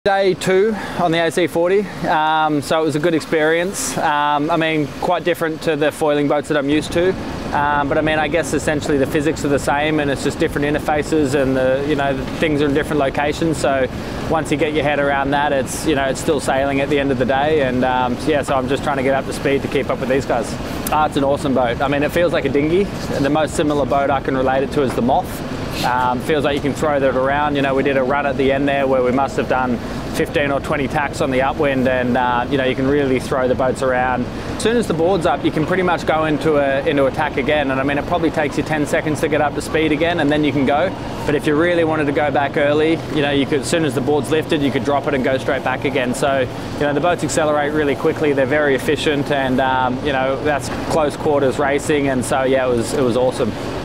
First Impressions of the AC40
Kyle Langford is impressed after just 2 days on the NYYC American Magic AC40

The 37th America's Cup will be Defended in Barcelona by four-time America's Cup winner, the Royal New Zealand Yacht Squadron represented by their team, Emirates Team New Zealand.